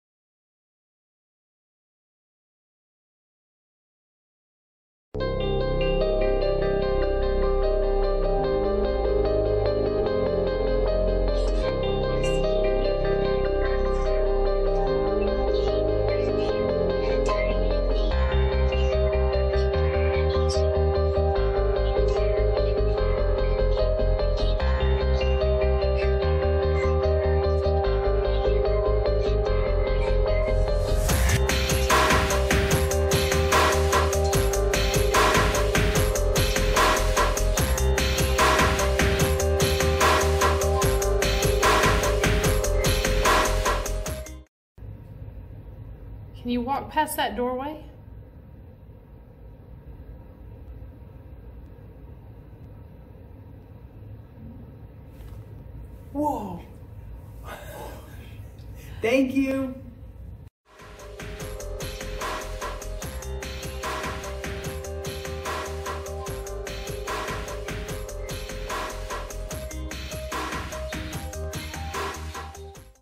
EVP during day tour of Ft. Mifflin
Collaborative quick clip with Ghost Biker Explorations; captured at Ft. Mifflin during a brief day tour.